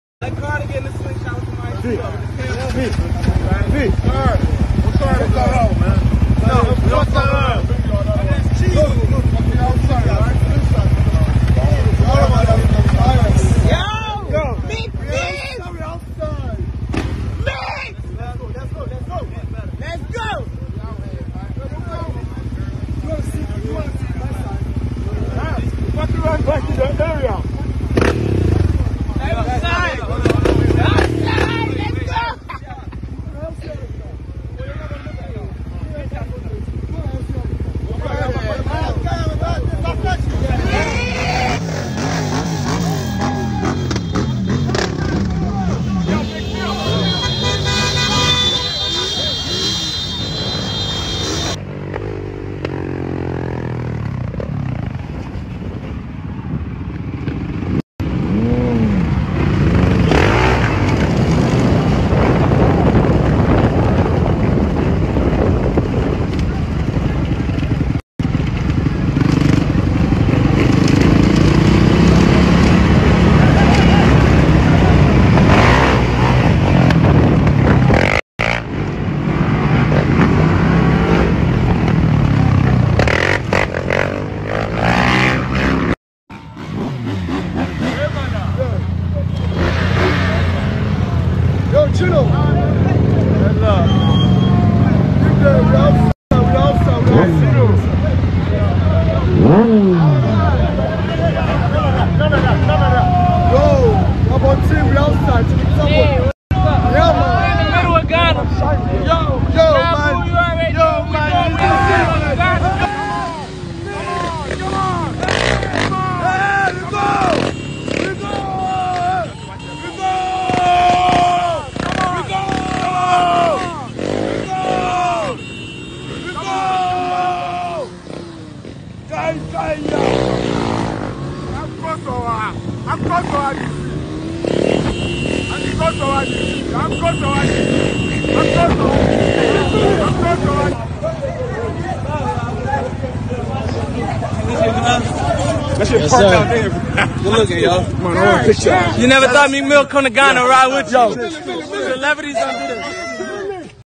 Meek Mill riding bike in the streets of Accra, Ghana 🇬🇭
Ahead of his performance at afronation, Meek Mill was spotted biking with the DC bikers in the streets Accra Ghana.